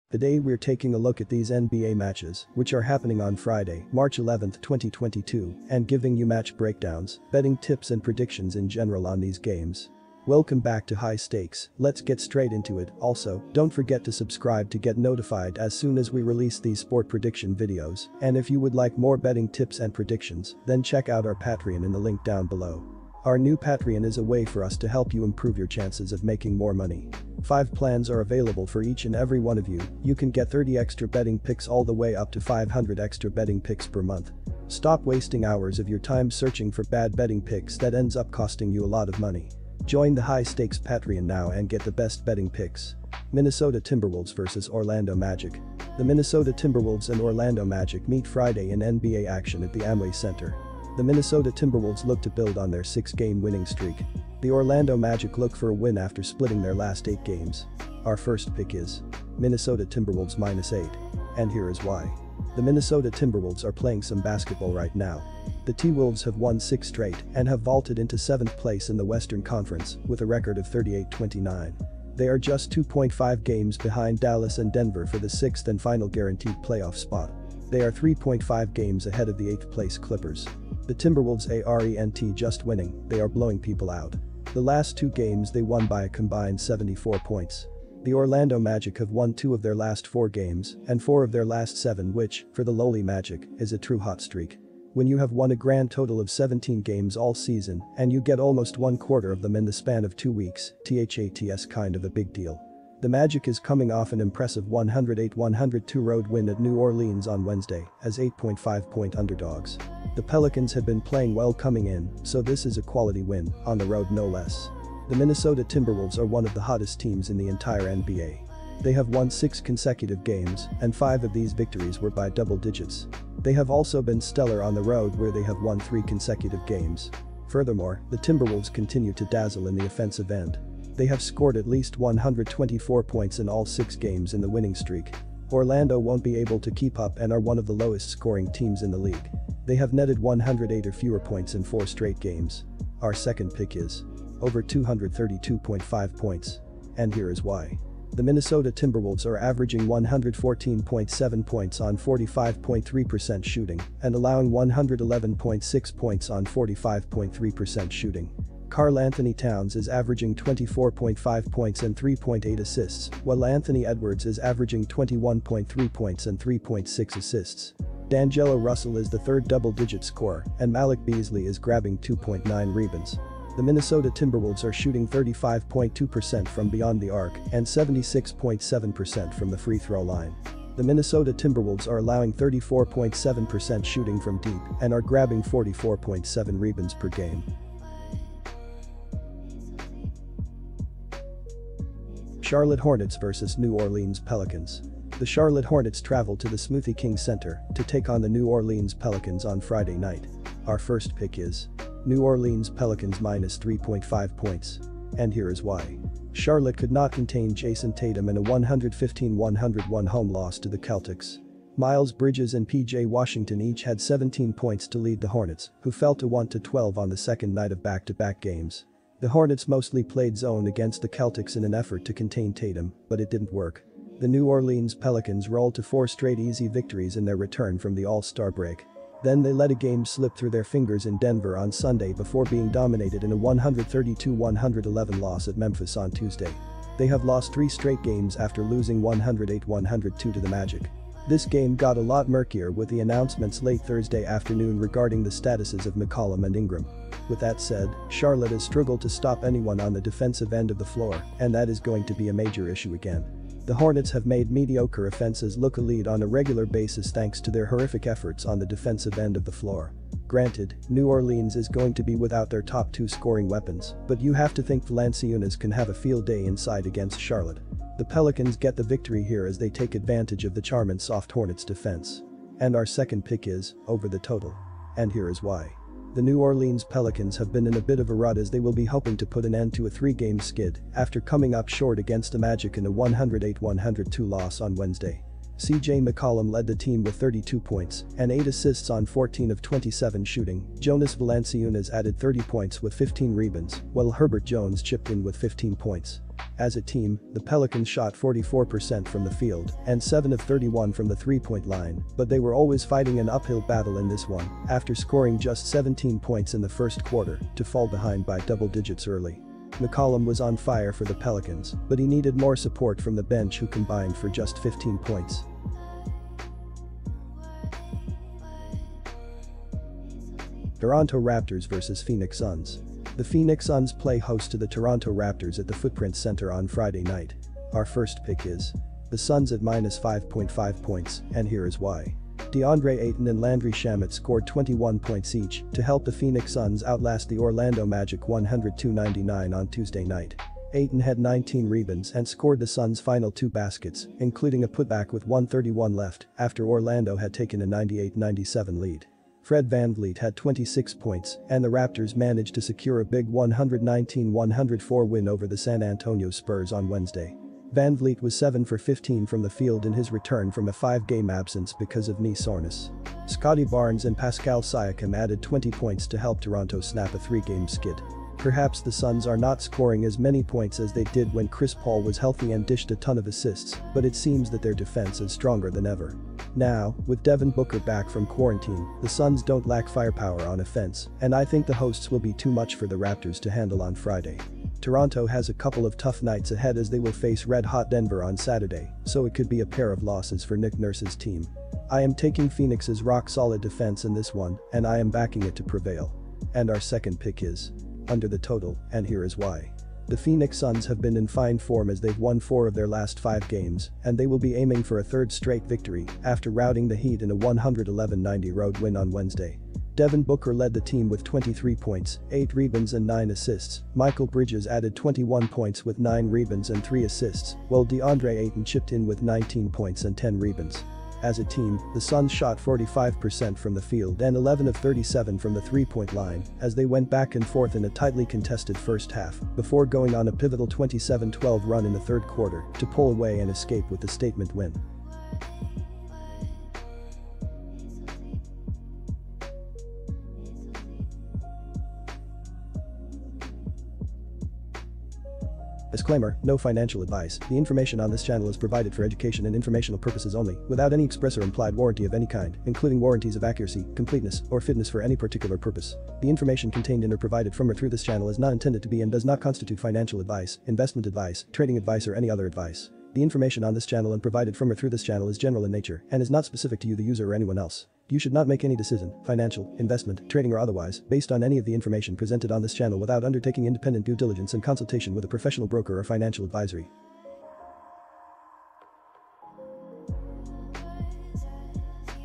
FREE NBA 3/11/22 Picks and Predictions Today Basketball Betting Tips and Analysis
Minnesota Timberwolves vs Orlando Magic
Charlotte Hornets vs New Orleans Pelicans
Toronto Raptors vs Phoenix Suns